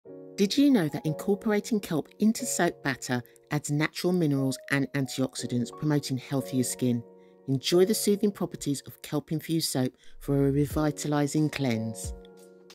Kelp is an awesome ingredient for soaps.
Did you know that Kelp is an amazing natural mineral?🤔

It is abundant in antioxidants, which help to promote healthy looking skin.🧖🏽‍♀️

When infused into soaps, it allows you to experience a spa like revitalizing cleanse.

You can check out our luxurious Mint, Green Clay and Kelp Soap bar for an ideal pamper treat. 🧼